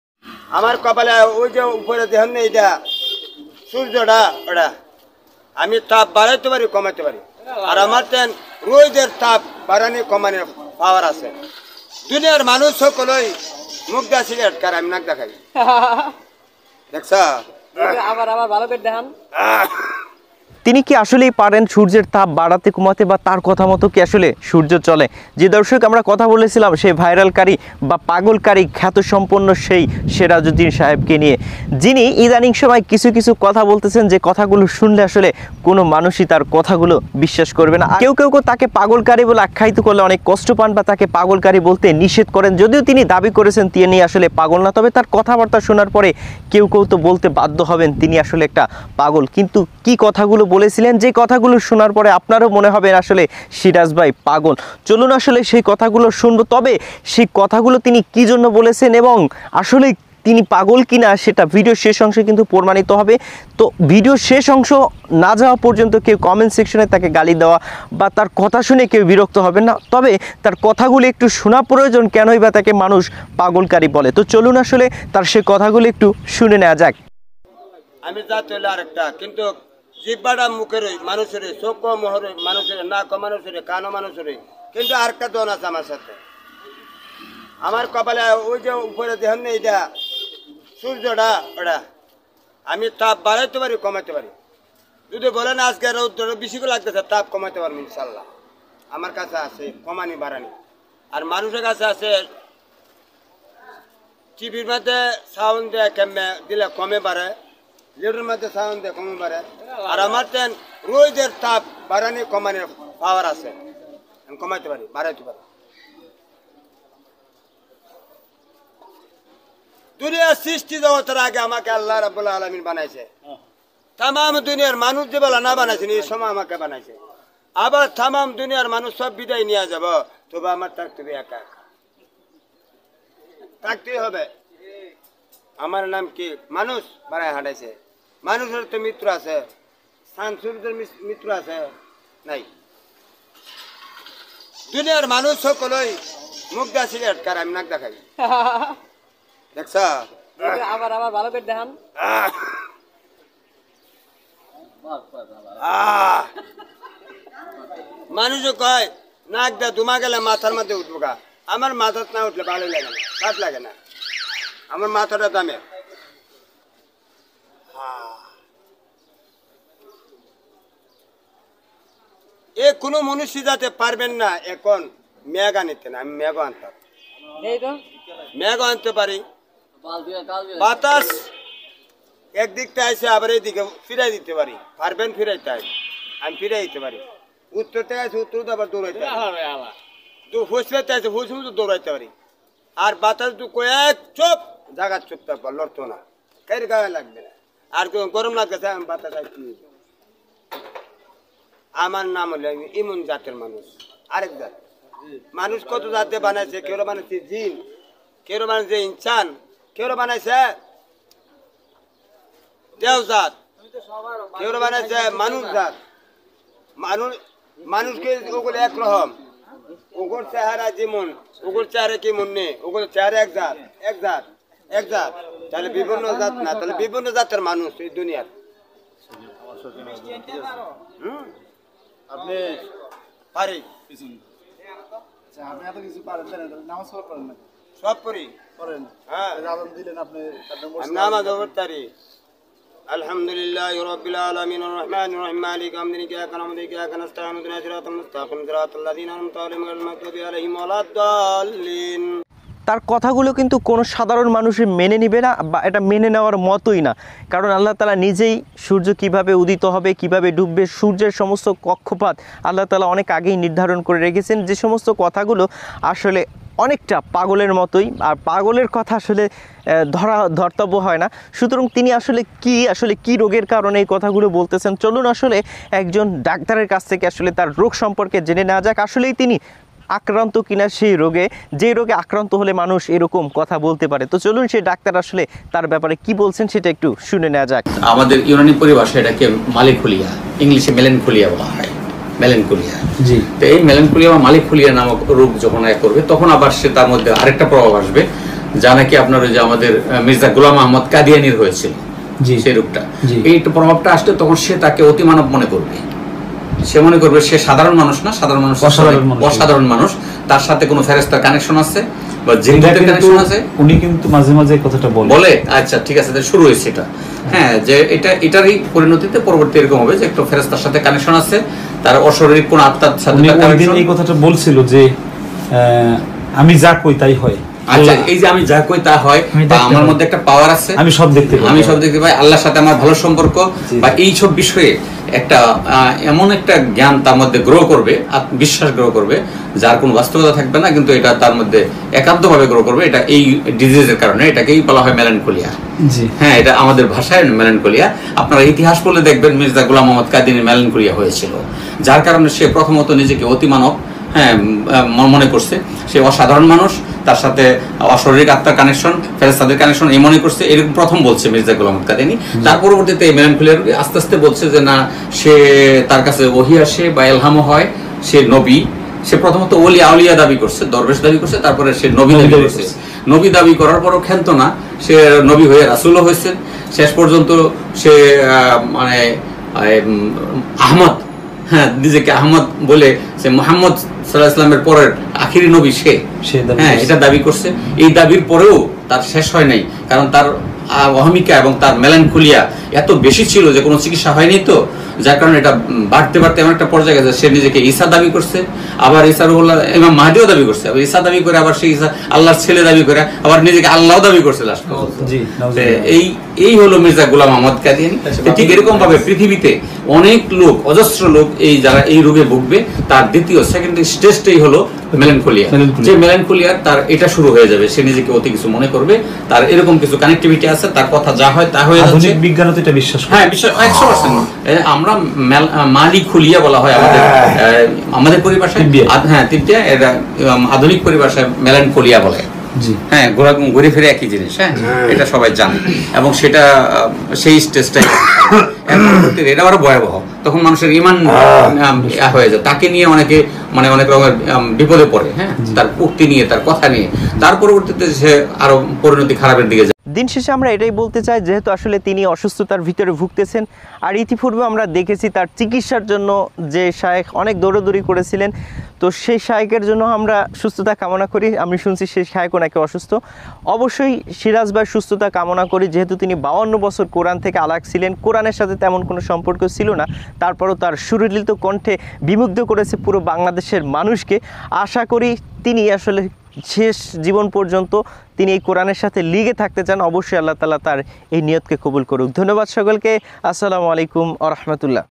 পাগল ক্বারী এসব কি বলে || সূর্যের তাপ আমি বাড়াতে কমাতে পারি || Vairal Kari || Pagol Seraj Kari
Since the viral recitation of the Qur'an by Mr. Siraj Uddin, who has a viral curry or insane reputation, has been at the top of criticism and discussion since the recitation of the Qur'an went viral.  He admits to his poverty, but the Qur'an is his and the Qur'an's happy address to return to him. He has taken responsibility for the treatment of his illness. Many people around him have also had porn for the rest of their lives, but some of his words are easy to prove crazy.  Those words will prove that he is really crazy